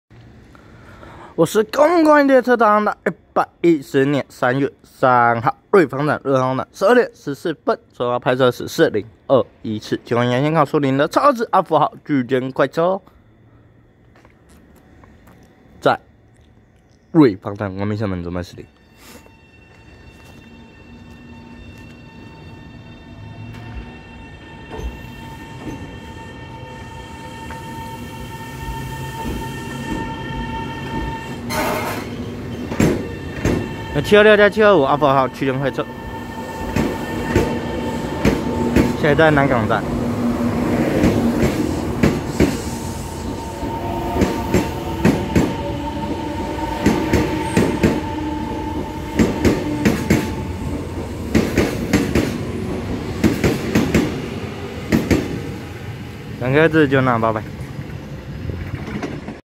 4021次經由宜蘭線開往樹林的EMU726+EMU725超優質阿福號區間快車
110/3/3瑞芳站12:14分拍攝4021次經由宜蘭線開往樹林的EMU726+EMU725超優質阿福號區間快車